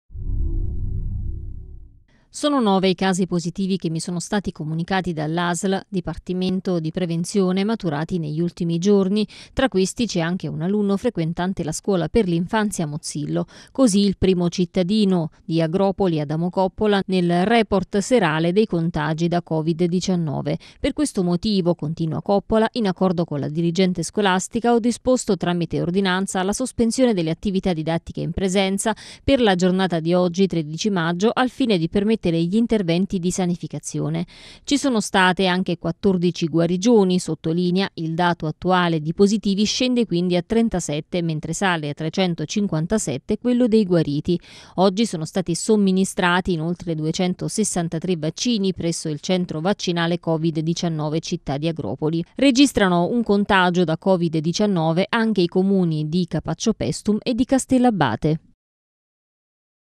Agropoli, 9 nuovi cittadini positivi: C’è anche un bimbo della scuola dell’infanzia Mozzillo
Sono 9 i casi positivi che mi sono stati comunicati all’Asl – Dipartimento di Prevenzione, maturati negli ultimi giorni. Tra questi c’è anche un alunno frequentante la Scuola per l’Infanzia “Mozzillo”.